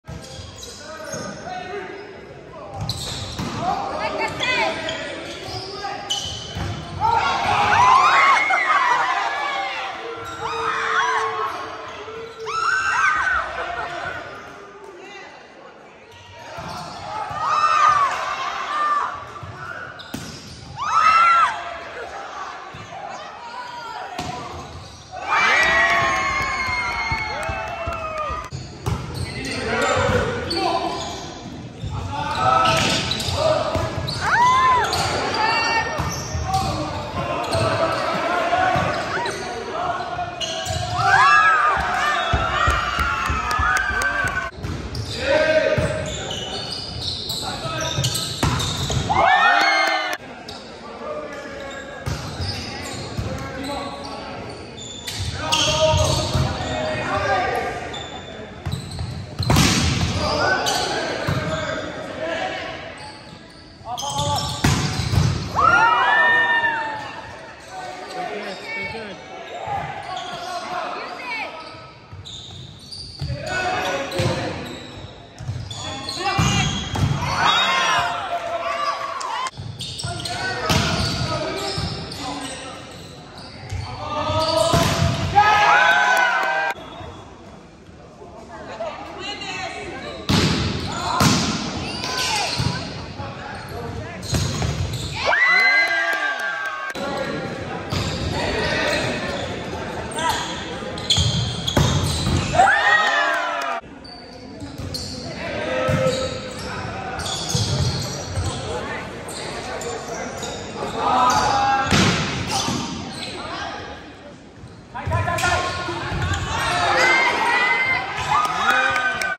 Wildcats vs Mustangs 2023 | highlights
Varsity Volleyball 2023 playoff
Whitney High School vs Monterey Trail High School
Whitney High School takes the win.